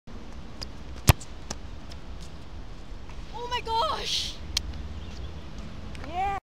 RLC17_24: Best Shot So Far... pt. 2 (freekickerz Top 5 Goals of the Week)
IMPORTANT UPDATE!!! IM ON THE HONG KONG NATIONAL TEAM FOR THE GIRLS U16 SOCCER/FOOTBALL TEAM!!!
yea i really want this to get in, considering the distance is great, dip, problem is power. so yea.